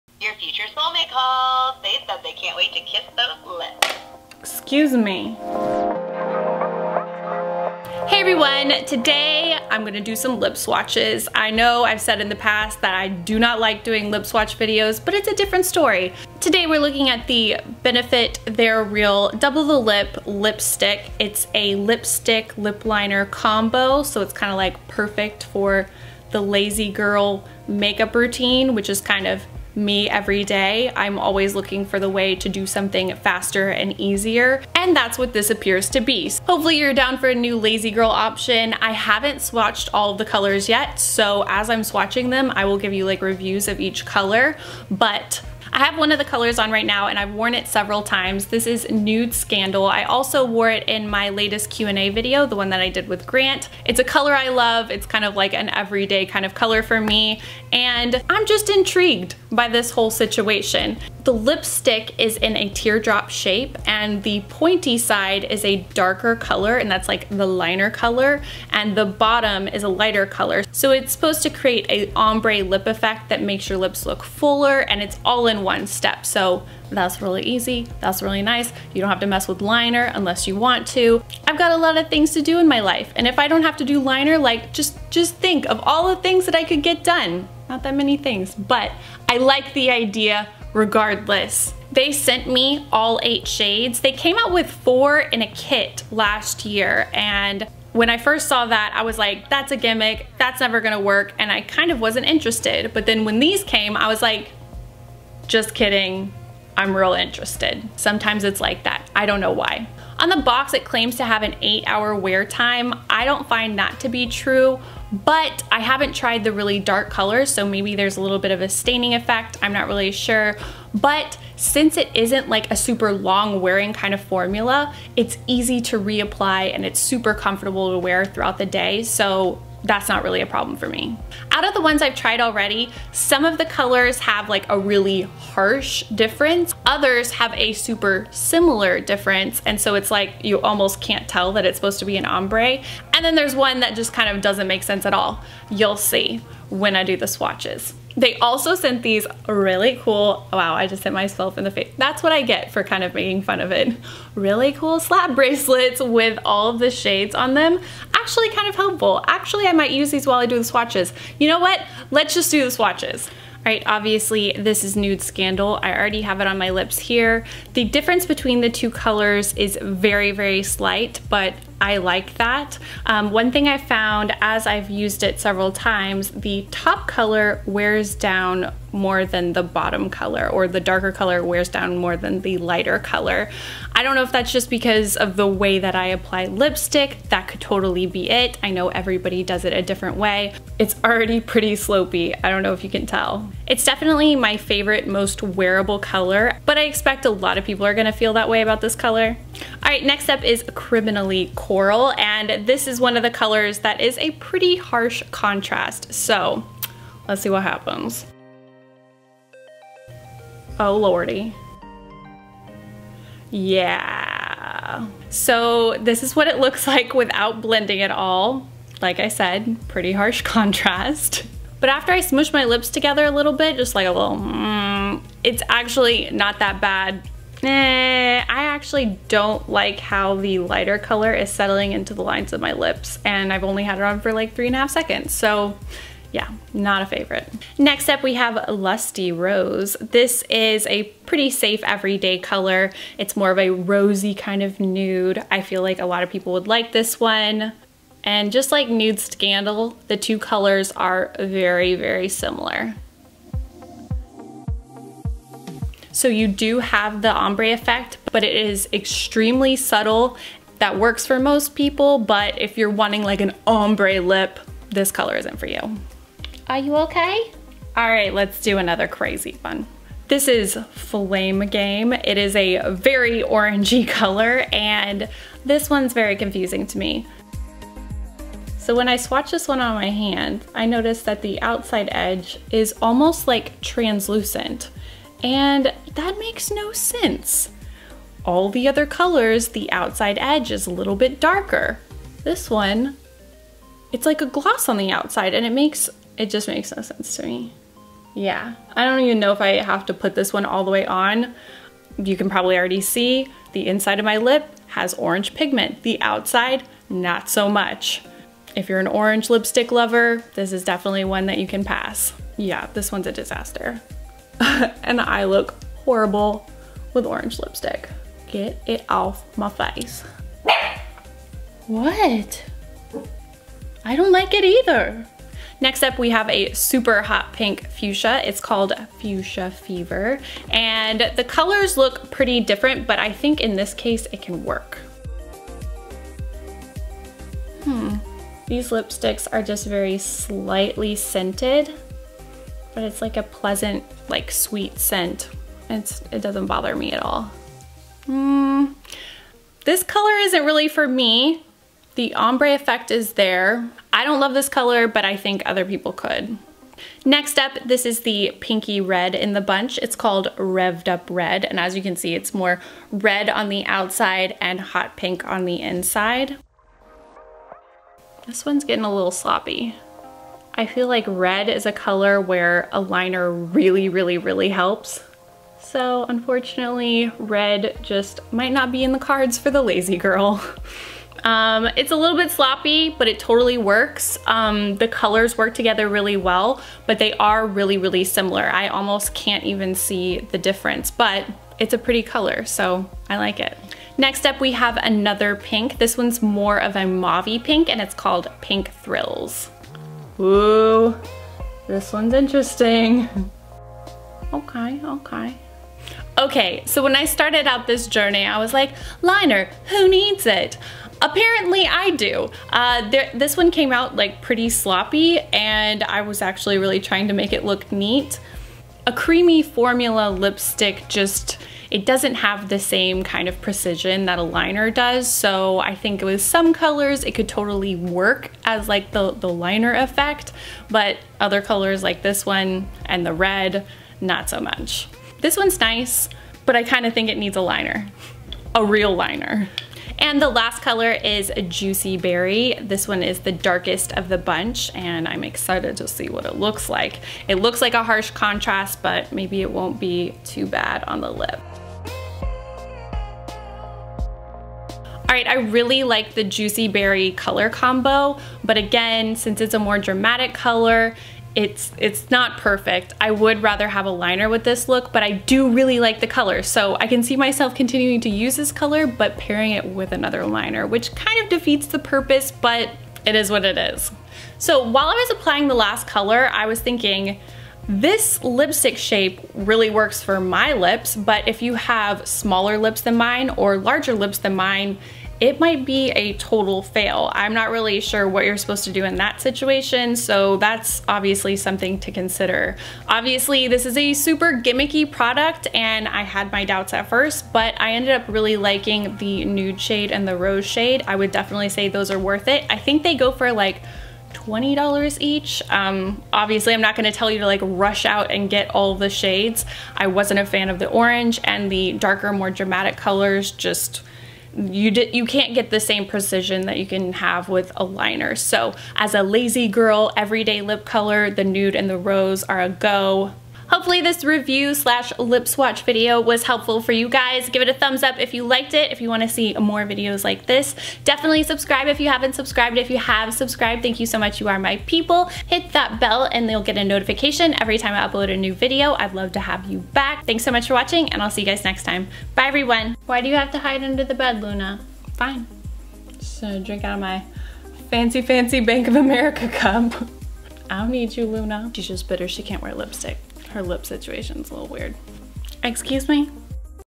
BEST LAZY GIRL LIPSTICK? | REVIEW + SWATCHES BENEFIT DOUBLE THE LIP | LeighAnnSays
Benefit release 8 shades of the They're real! double the lip lipstick & liner in one. Kind of gimmicky but it could be possibly be the best lazy girl lipstick ever. I did lip swatches and reviewed each shade so you dont have to :)